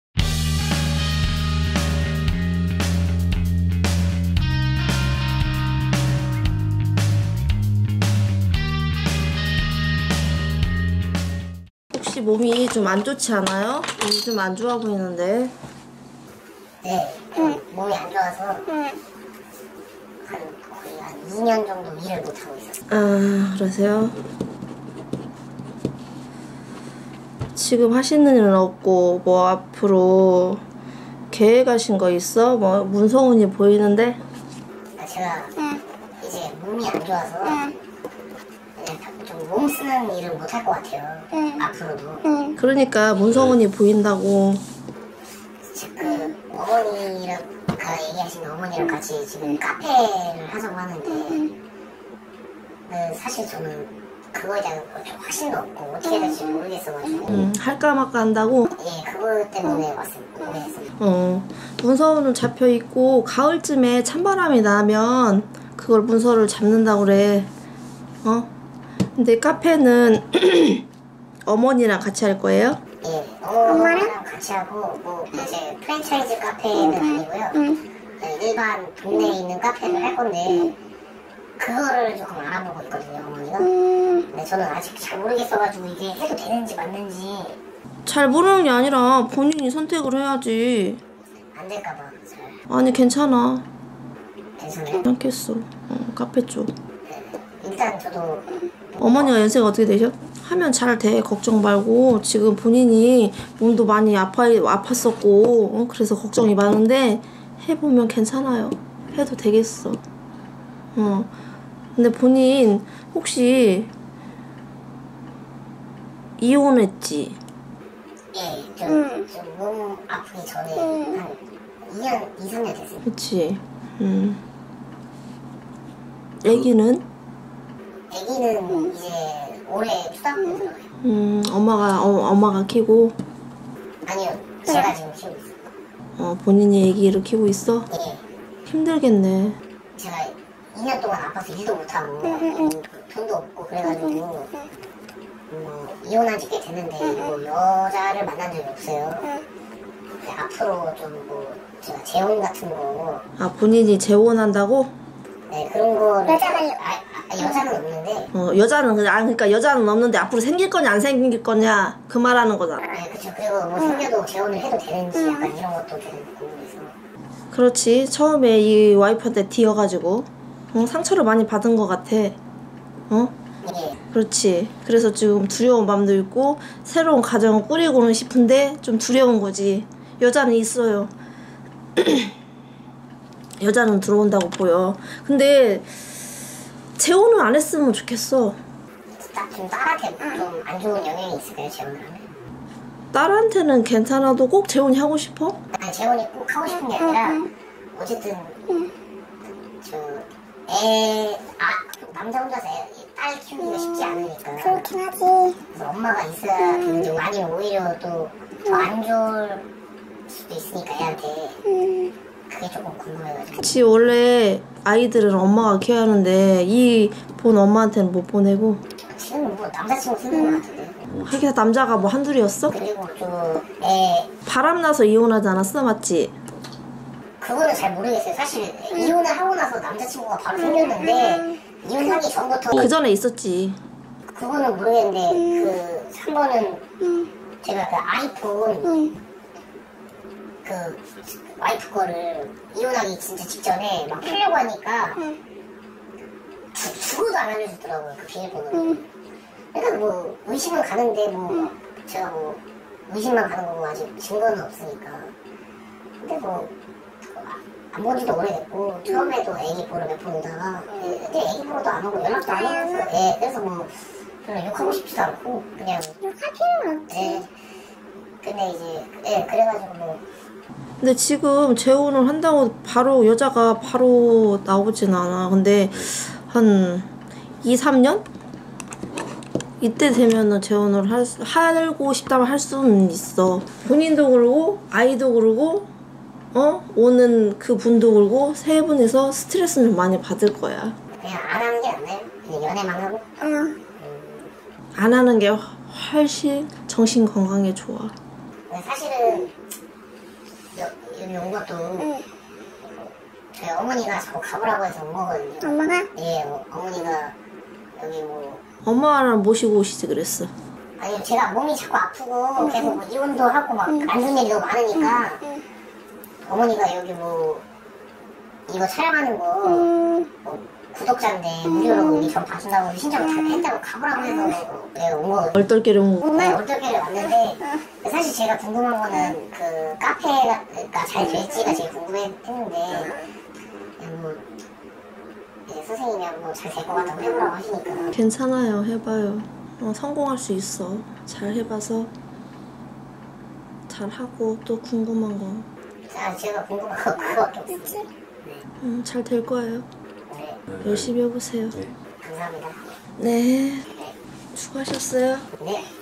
[천신매화당] 아내가 바람펴서 이혼하고 초등학생 딸을 혼자 키우는 아버지 응원합니다 [구독자 점사]
대한민국 NO.1 무속인 유튜브 [신과함께 민필름]

촬영문의) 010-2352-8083 (제작사 민필름)